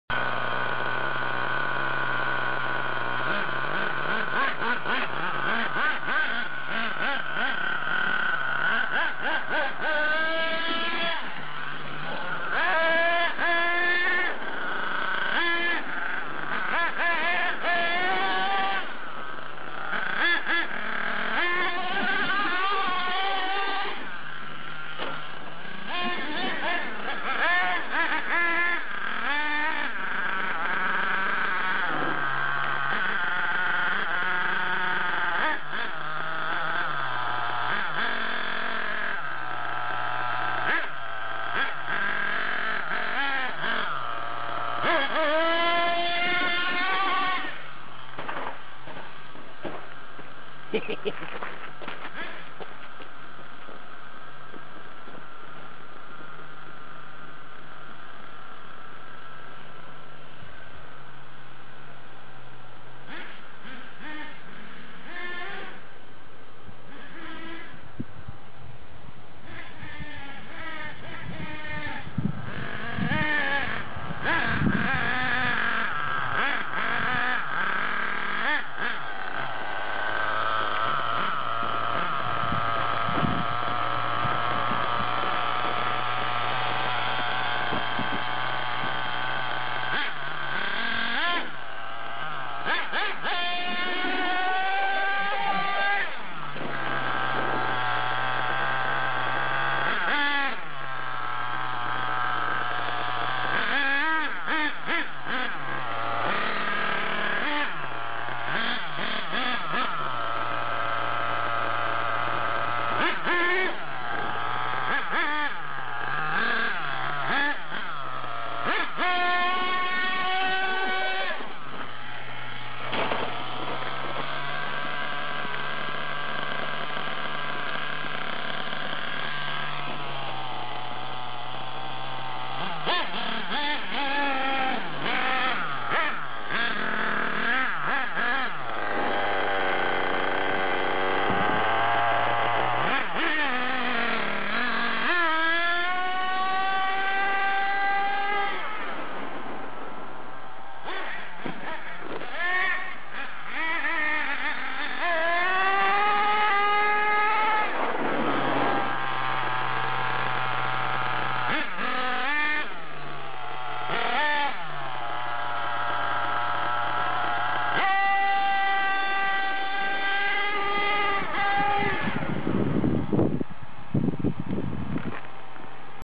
More MGT Driving
Just some driving. Cracked all of the body pins in this short run :)